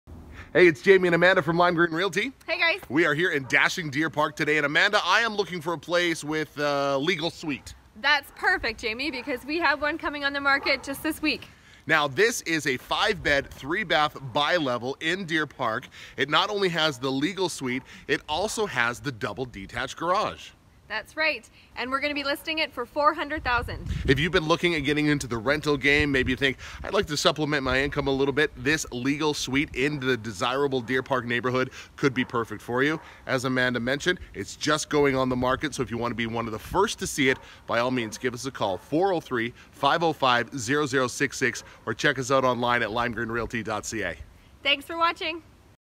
Red Deer House for Sale - 138 Duval Cres - Red Deer
Amanda and Jamie announce a new Lime Green Realty listing located at 138 Duval Cres. This one has a legal suite and a sweet price!